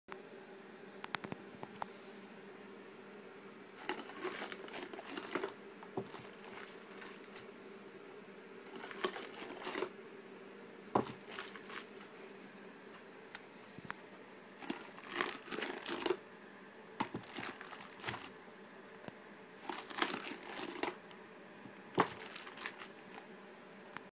Cool masking tape packing
Simple things please simple minds. The new rolls of masking tape here at AVR and their packaging make a really pleasing sound and action when lifted...